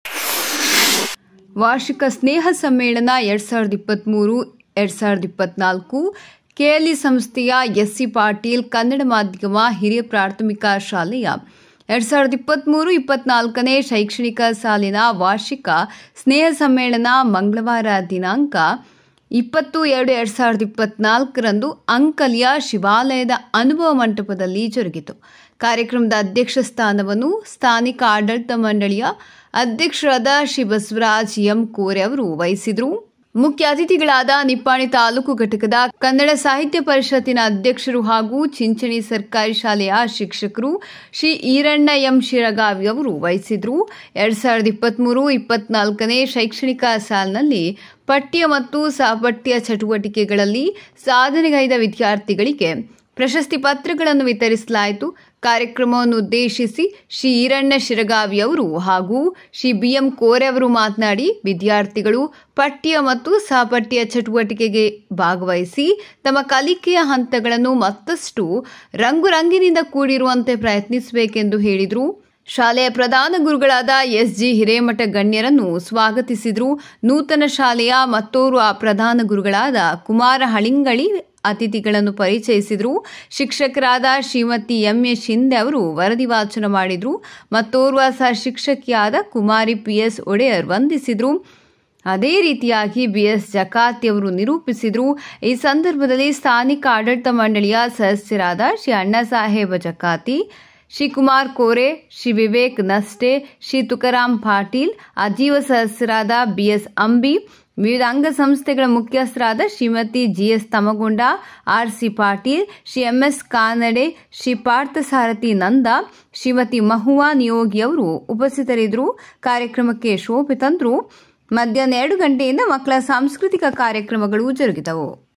ಅಂಕಲಿ : KLE ಸಂಸ್ಥೆಯ ಎಸ್ ಸಿ ಪಾಟೀಲ ಕನ್ನಡ ಮಾಧ್ಯಮ ಶಾಲೆಯ 2023-2024ನೇ ಸಾಲಿನ ವಾರ್ಷಿಕ ಸ್ನೇಹ ಸಮ್ಮೇಳನ
ಅಂಕಲಿ : KLE ಸಂಸ್ಥೆಯ ಎಸ್ ಸಿ ಪಾಟೀಲ ಕನ್ನಡ ಮಾಧ್ಯಮ ಶಾಲೆಯ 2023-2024ನೇ ಸಾಲಿನ ವಾರ್ಷಿಕ ಸ್ನೇಹ ಸಮ್ಮೇಳನ (ICB NEWS ANKALI)